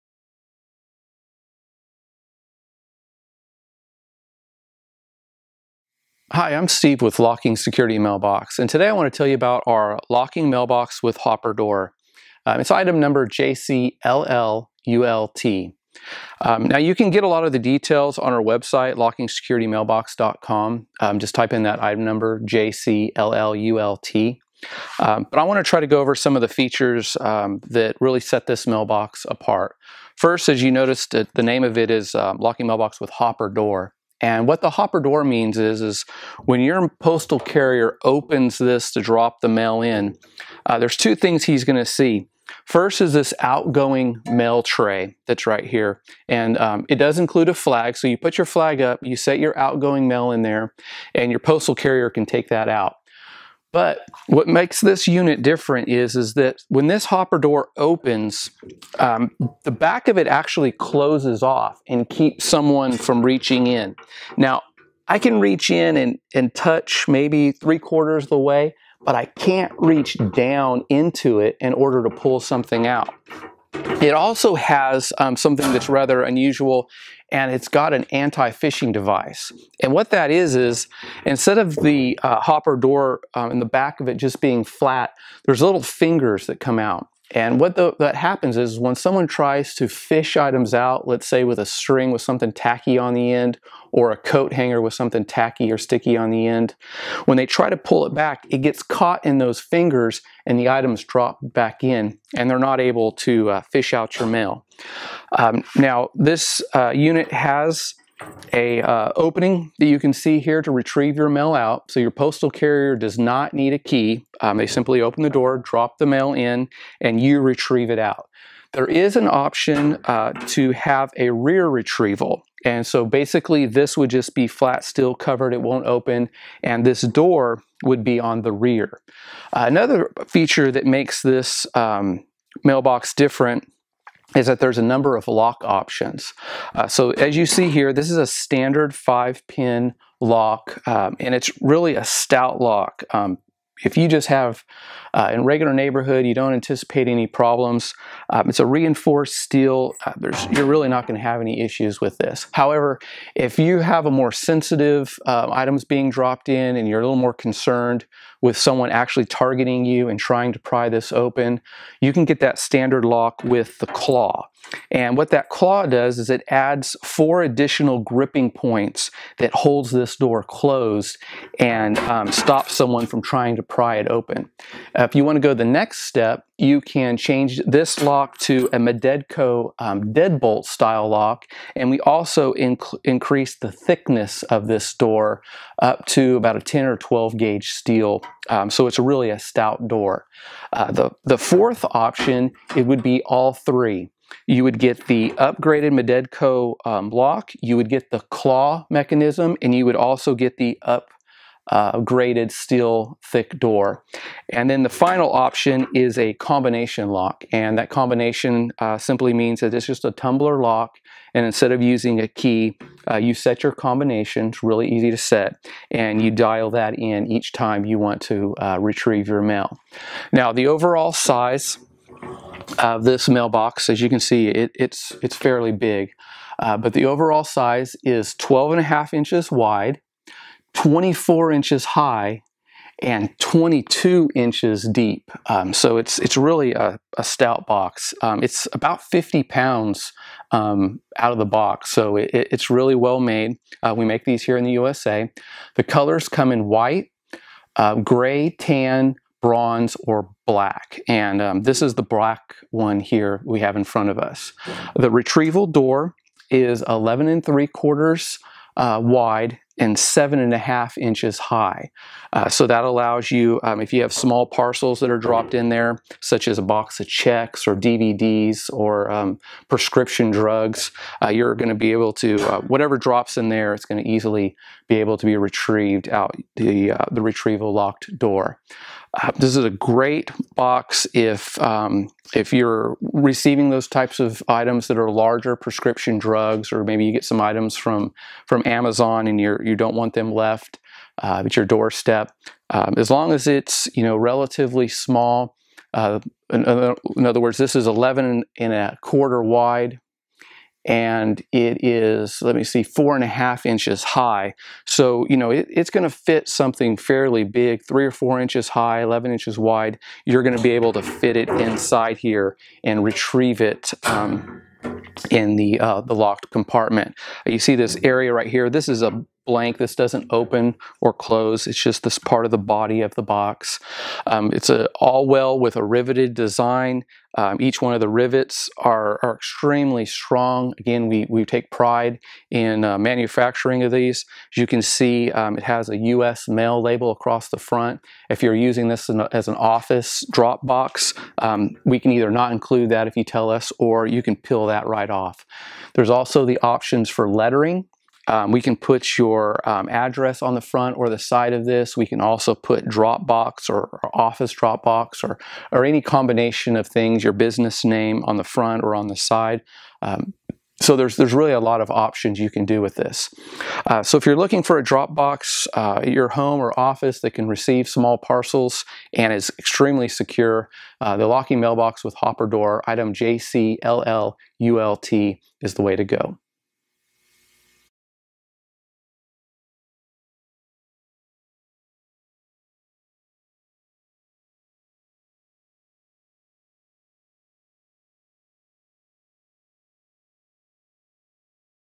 Locking Mailbox with Hopper Door Demo / Review
With a large hopper door to accept small parcels like prescription drugs, box of checks or DVD's this large mailbox will work for the home or office.   The hopper door opening is just over 11 inches wide and 4.5 inches high. The overall size of the mailbox is 12.5” W x 24” H x 22" D (including overhang).

There is an outgoing mail which is:  2"H x 10"W x 6"D, and it includes a flag that you install after delivery.  It comes standard with a durable high-grade 5 pin lock. There are several other lock options, which keep your mail safe from mail theft.

There are a number of lock options and an aluminum post can be added to the order.  It includes an “anti-fish” feature located at end of the hopper door. This helps prevent fishing which is a common method used by mail thieves.